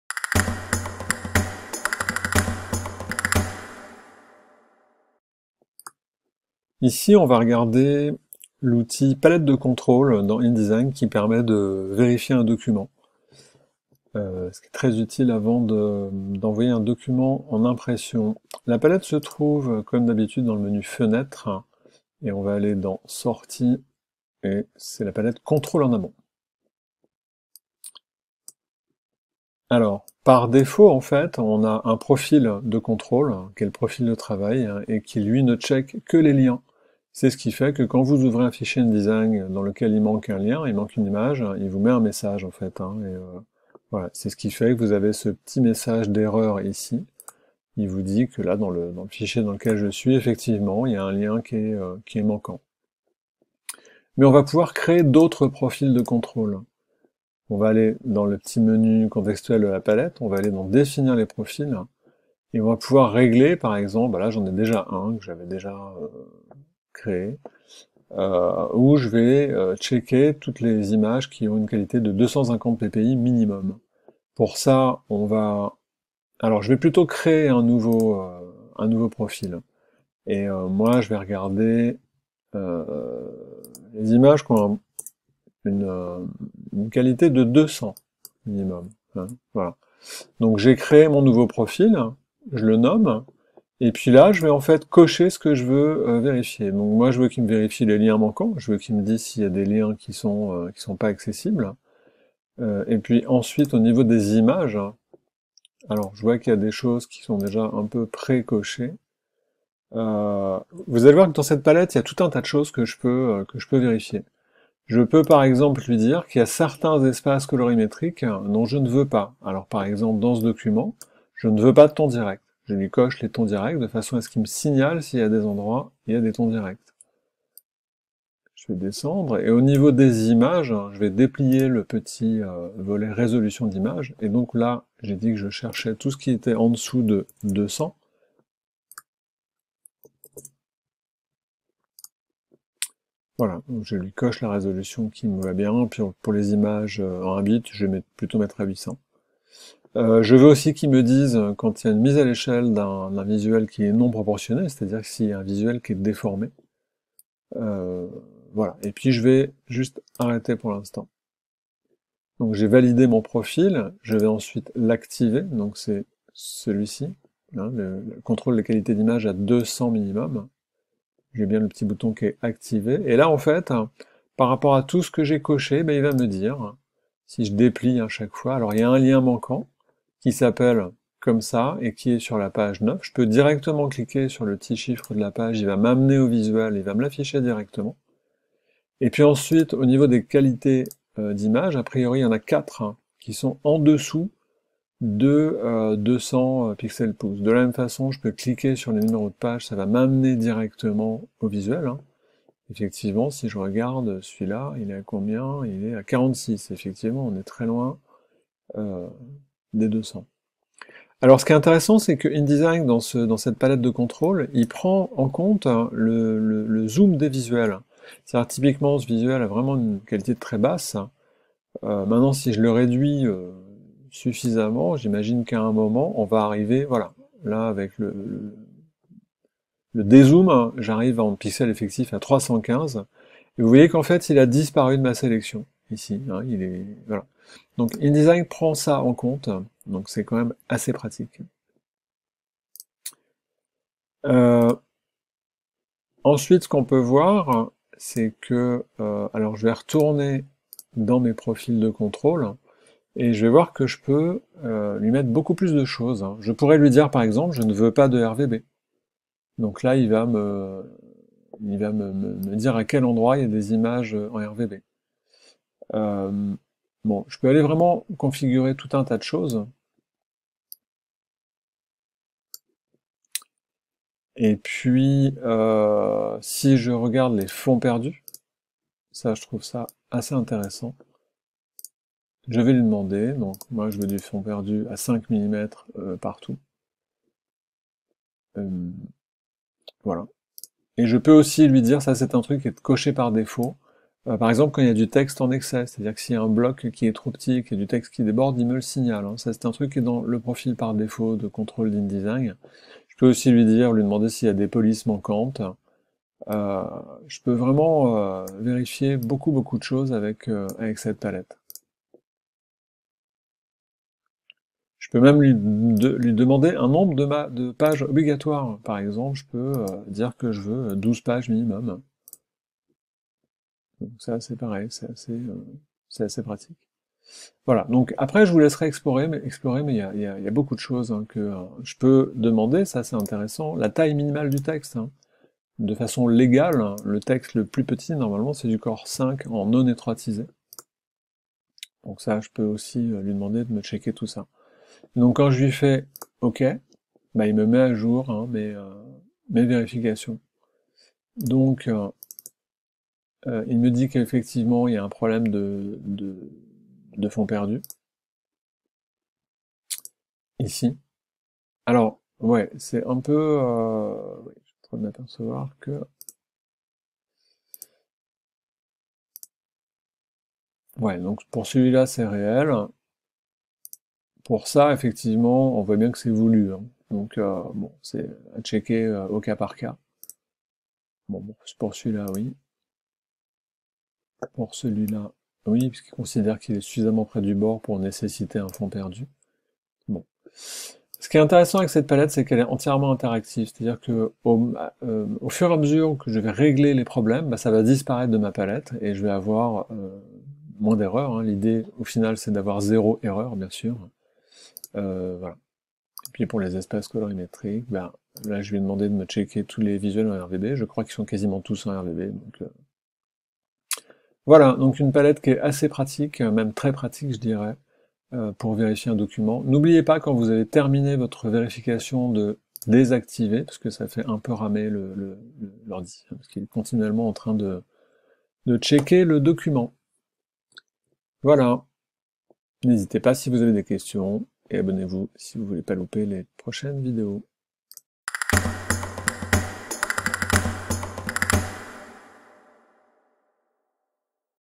InDesign Contrôler un fichier avant l'impression avec la palette de contrôle en amont
Ici on regarde comment faire la vérification d'un fichier avant le départ en impression, via la palette de contrôle en amont.

Ma chaîne YouTube déménage.